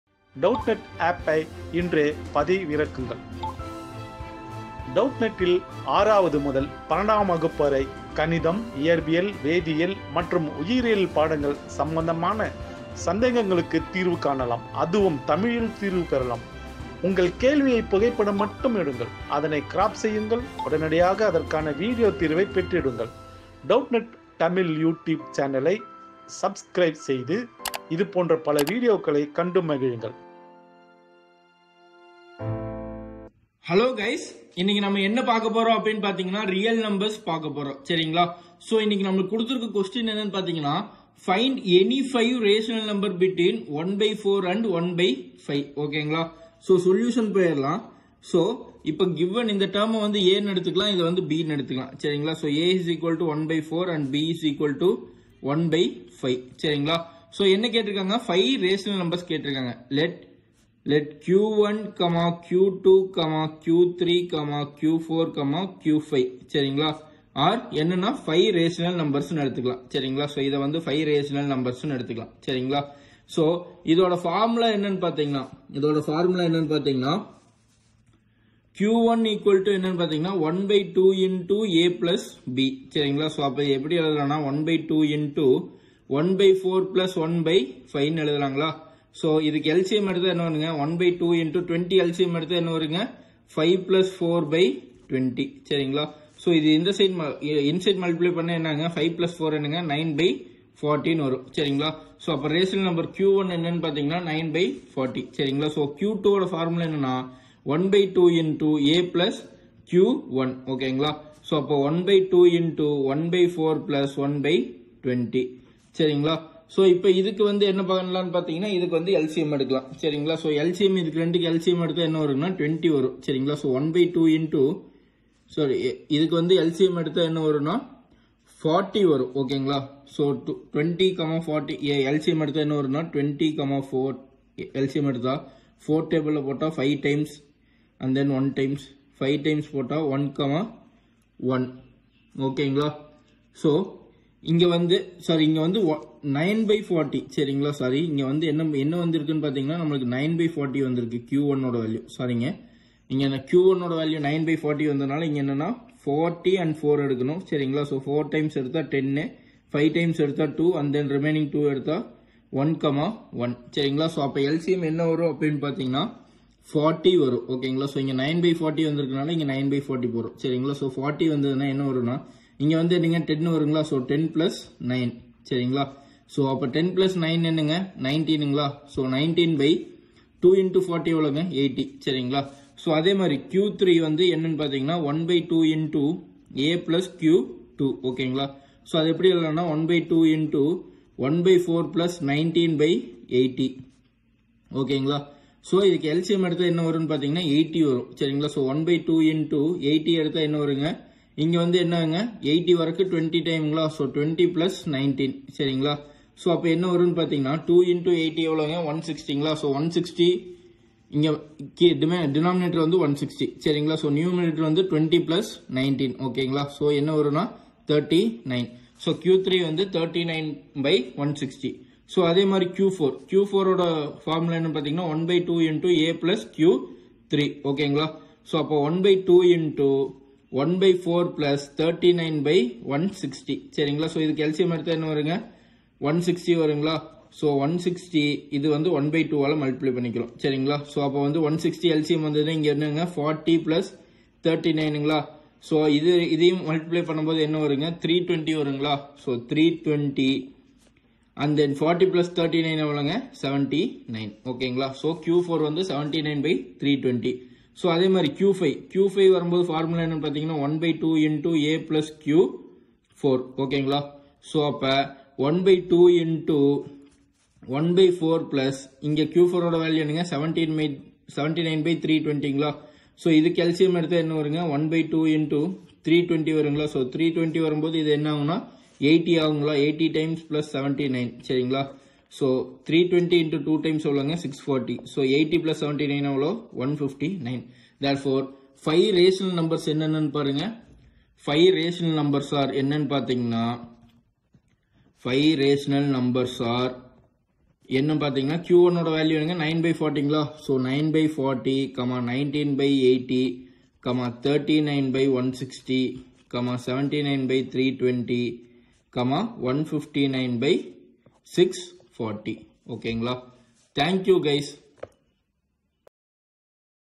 Find any five rational numbers between `1/4` and `1/5` | Class 9 Maths | Doubtnut
Find any five rational numbers between `1/4` and `1/5`

Online Class में Admission लेने के लिए यहाँ click करें :
👉 Class 9 | CBSE Boards | English Medium :

👉 Have Any Query? Ask Us.